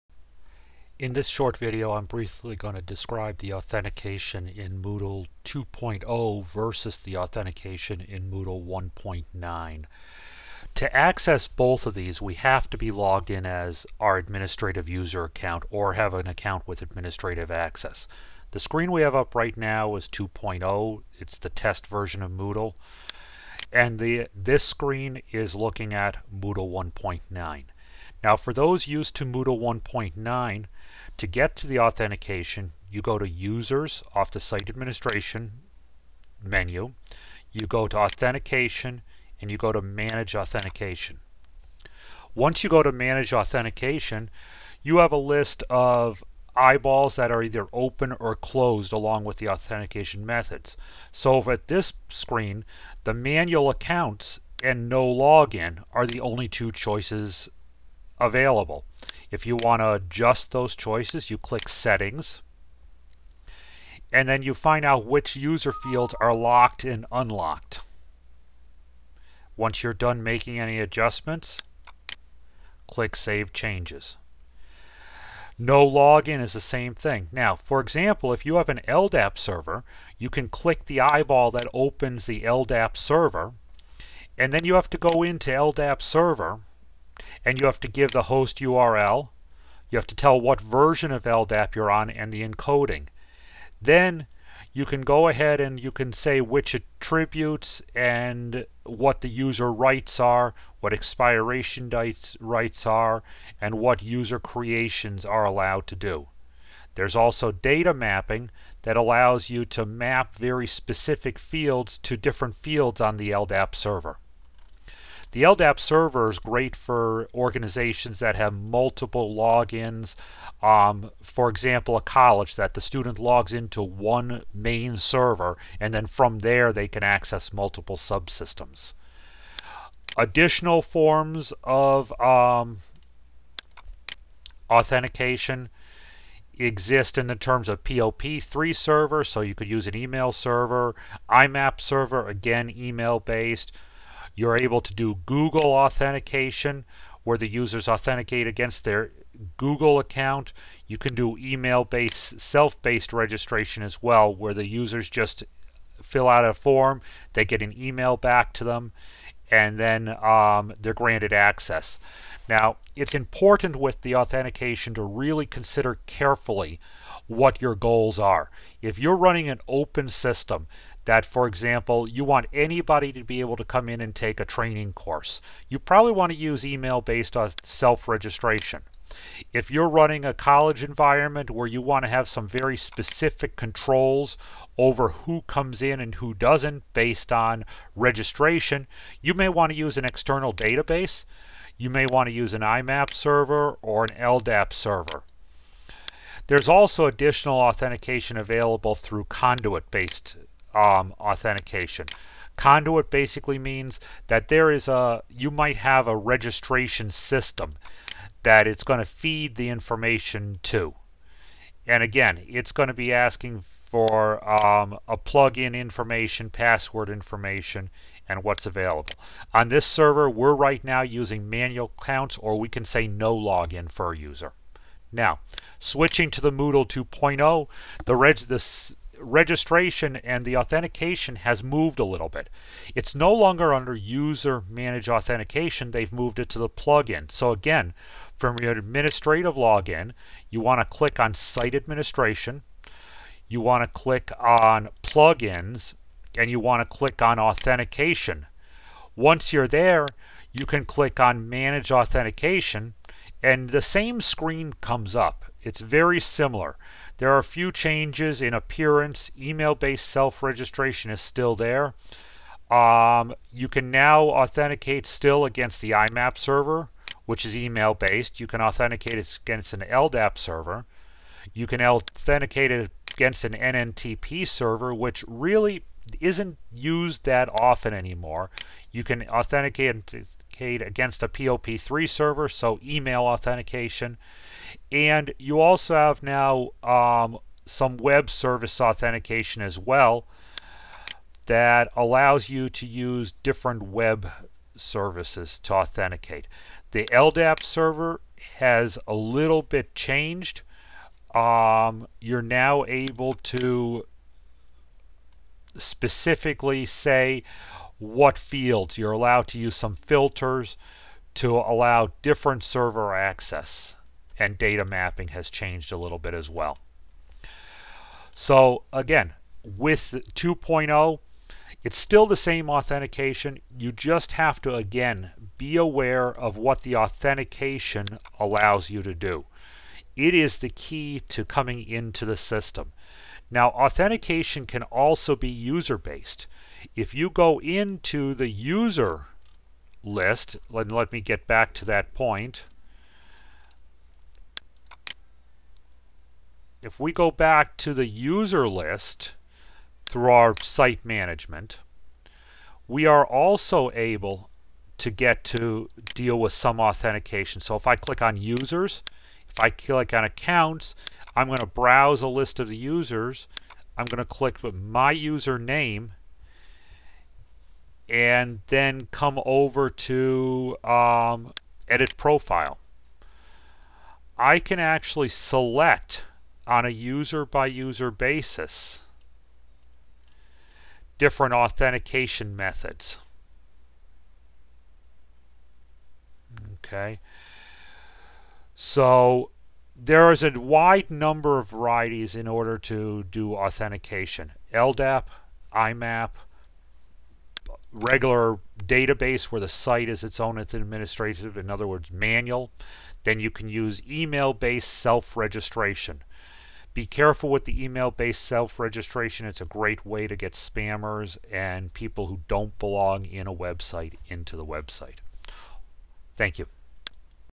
Moodle 200 User Authentication Moodle 2.0 to Moodle 1.99
The differences in User Authentication between Moodle 2.0 and 1.99